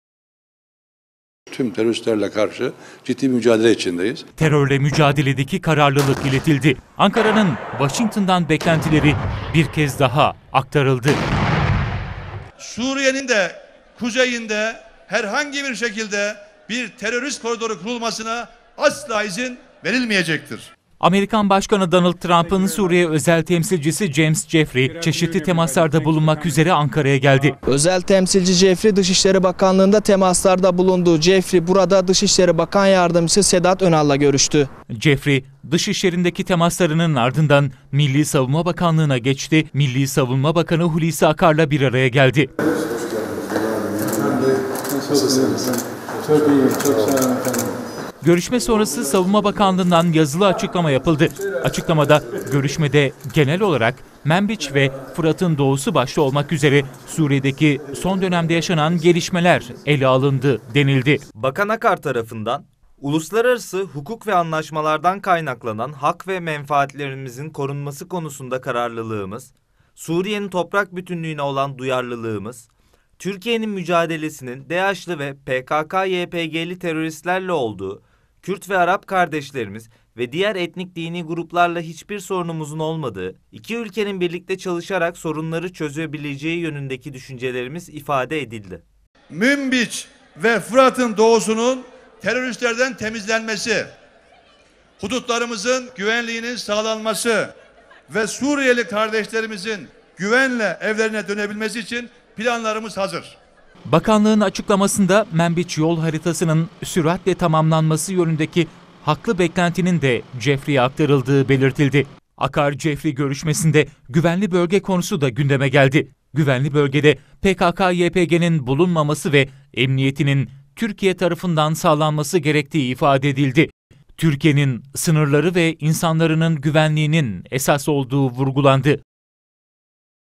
Hulusi Akar Jeffrey'yi kabul etti
Amerika'nın Suriye Özel Temsilcisi James Jeffrey ile görüşen Milli Savunma Bakanı Hulusi Akar, IŞİD ve PKK/YPG ile mücadelenin sürdüğünü vurguladı. Akar, güvenli bölgenin de Türkiye kontrolünde olması gerektiğini söyledi.